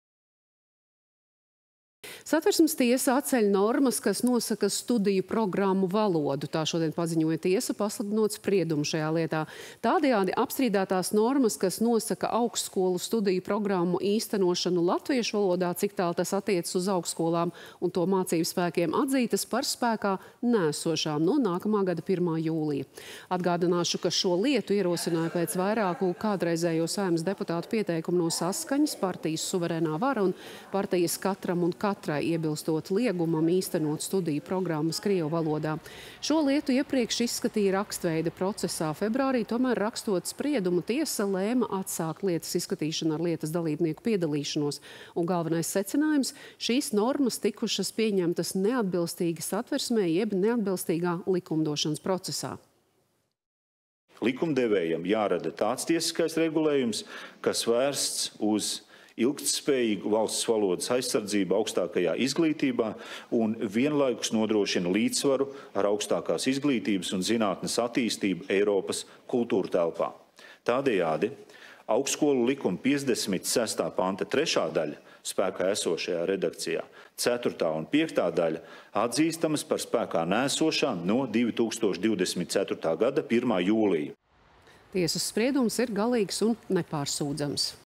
ST atceļ normas, kas nosaka studiju programmu valodu
Satversmes tiesa atceļ normas, kas nosaka studiju programmu valodu. Tā šodien paziņoja tiesa, pasludinot spriedumu šajā lietā. Tādējādi apstrīdētās normas, kas nosaka augstskolu studiju programmu īstenošanu latviešu valodā, ciktāl tas attiecas uz augstskolām un to mācībspēkiem, atzītas par spēkā neesošām no nākamā gada 1. jūlija.